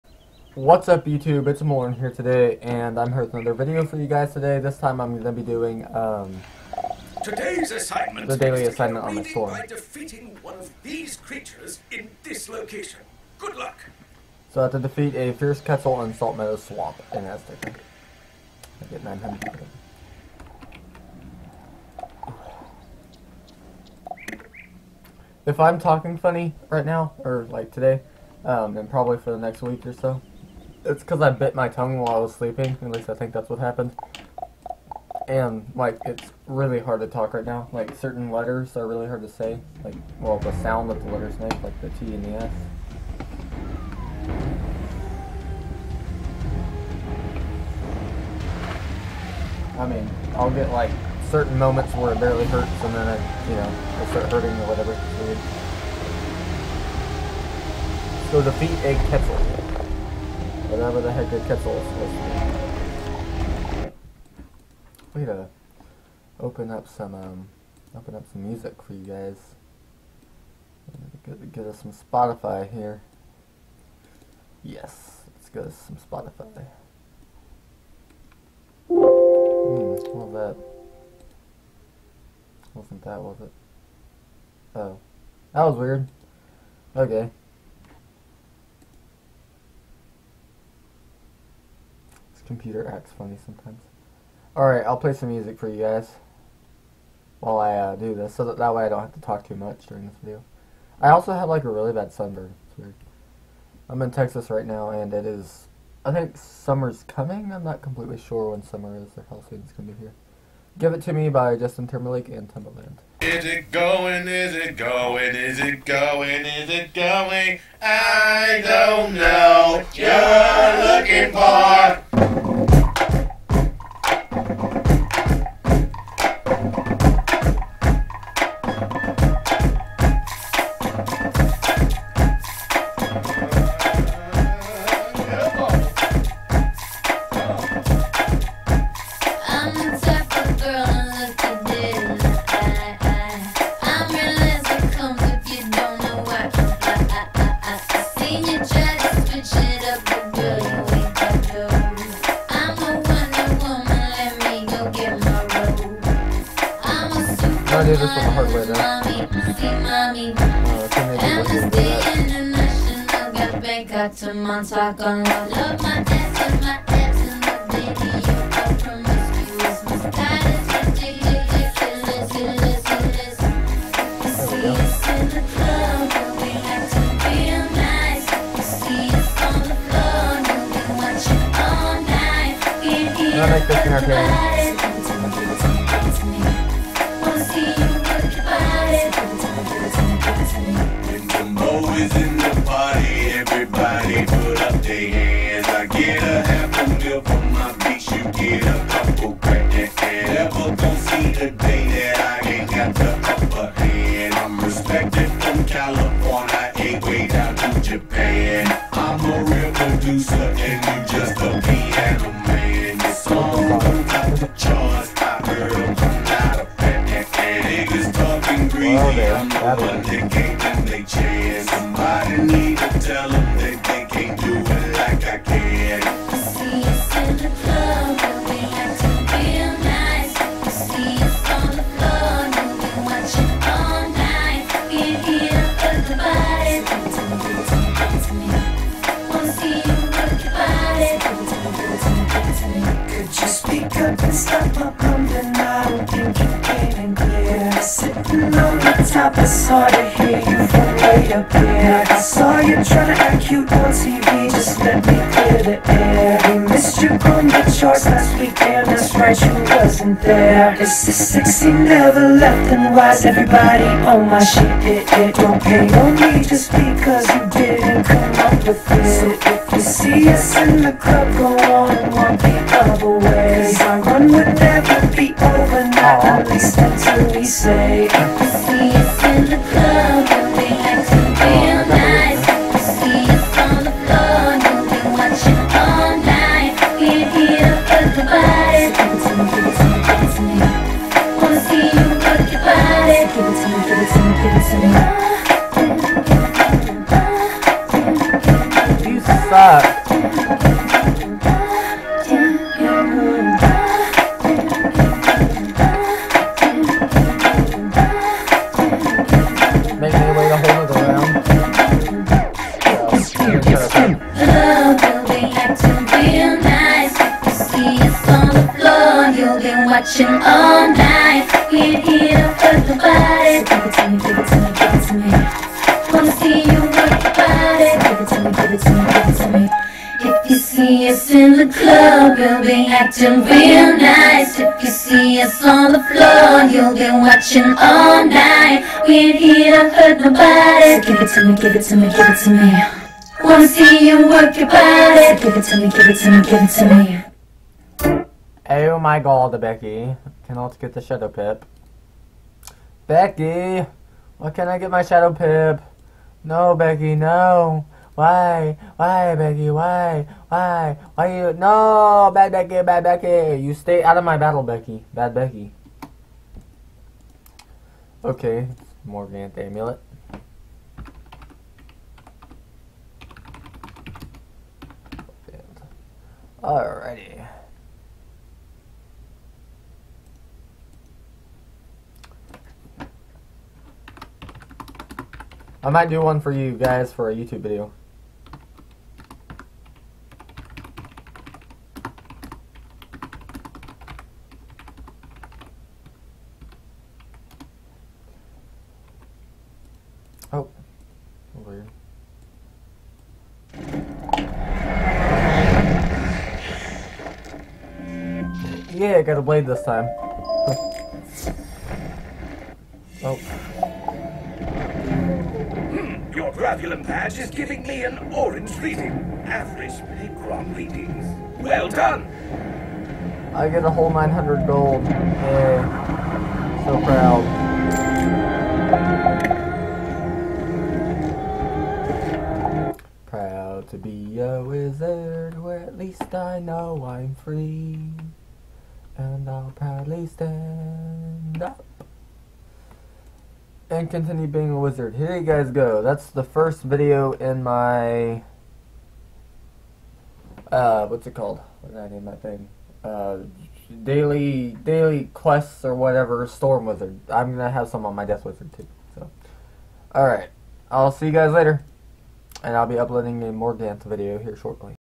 Daily Quests on Storm P1
Watch as I do a daily quest on my storm wizard for the daily quest reward. Will the readings be yellow? Will they be red? How about purple? Green maybe? Ooh pink...yes...I like pink.